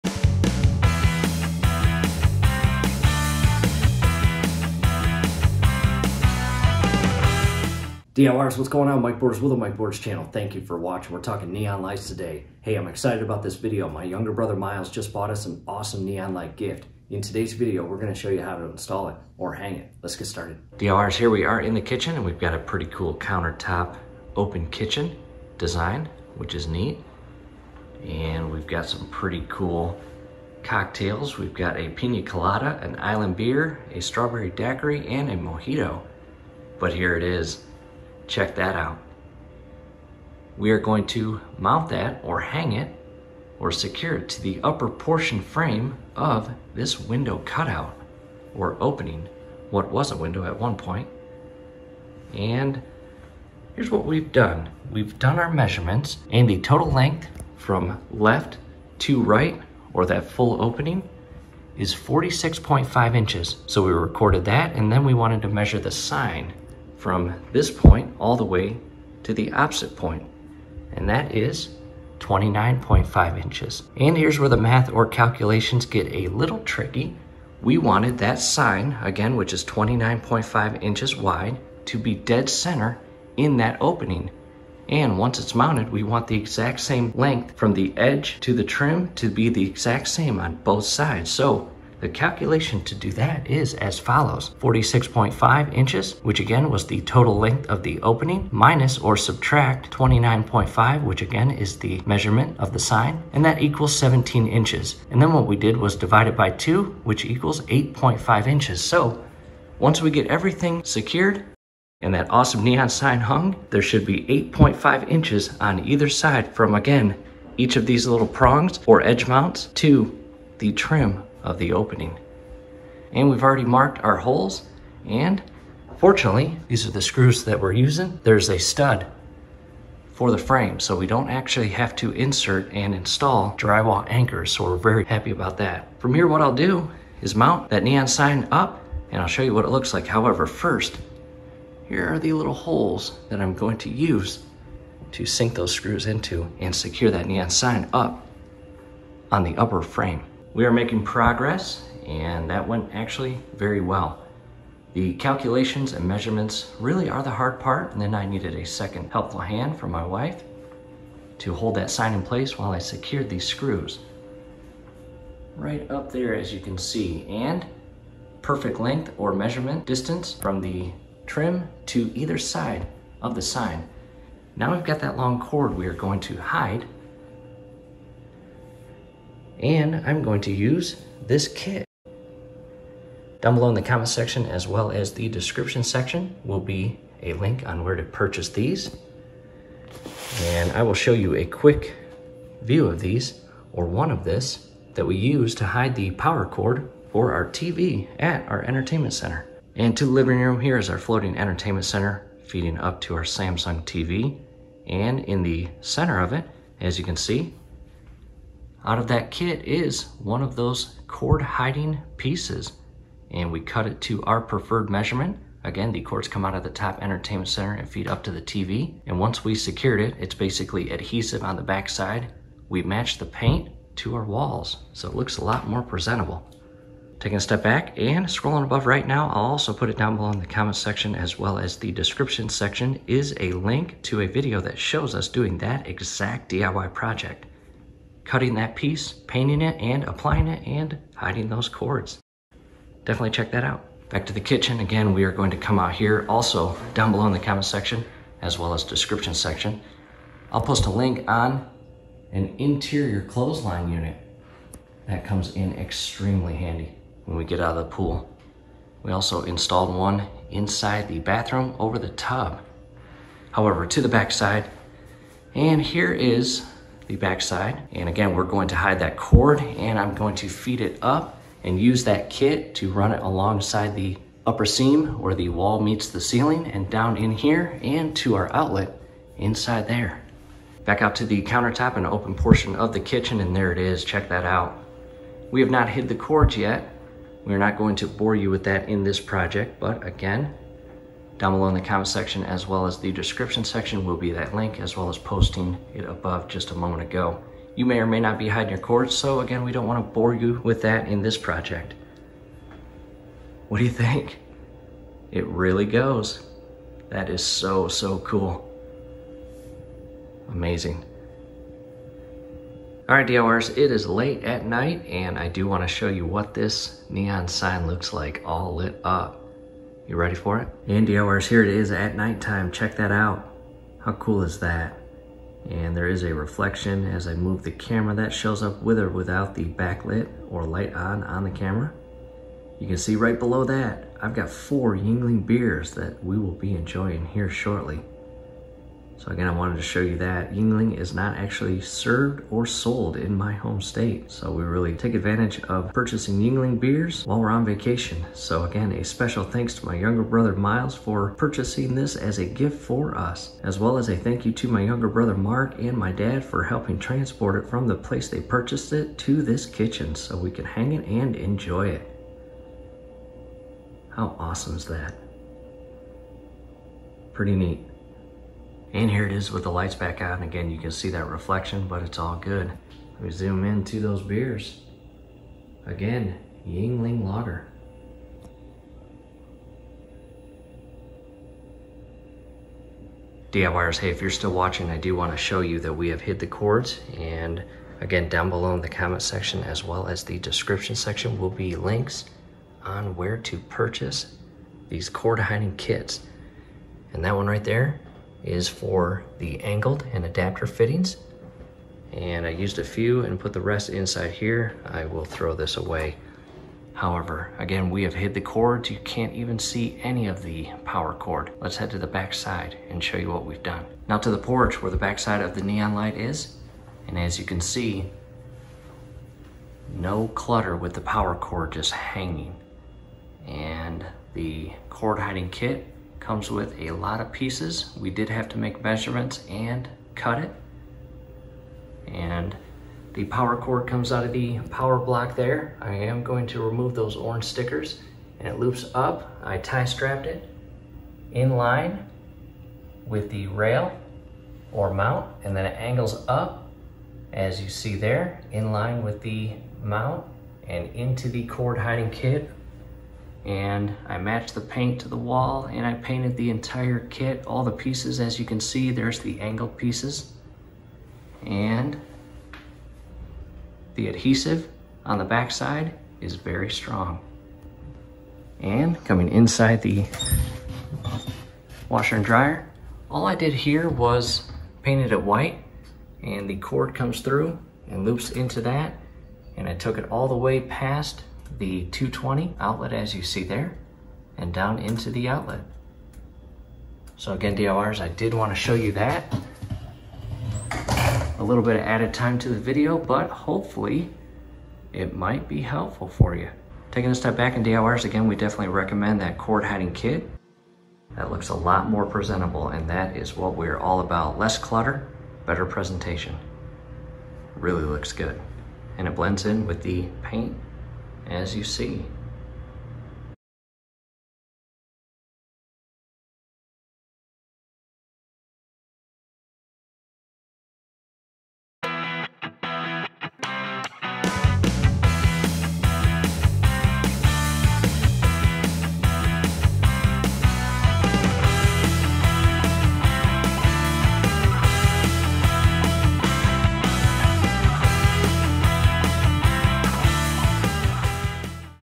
How to mount or hang neon sign properly and safely - DIY
Cord hiding kit

Cutting tool

Measure tape

Interior clothesline

Bathroom tub clothesline

1.  How to hide tv cords/cables

2.  Install clothesline above bathroom tub